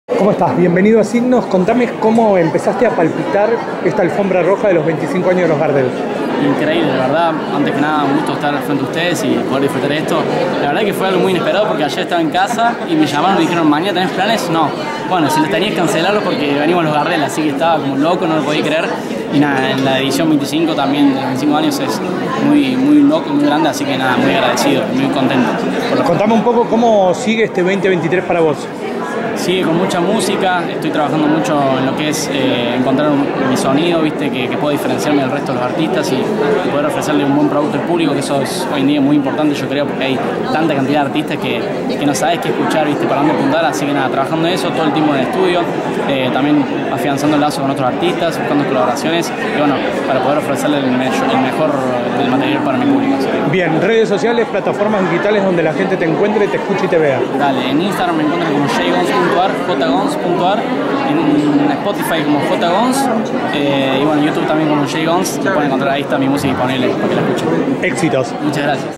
JGonz: "Alfombra roja de los Premios Gardel"
Presente en la alfombra roja de los 25AñosGardel en el movistararena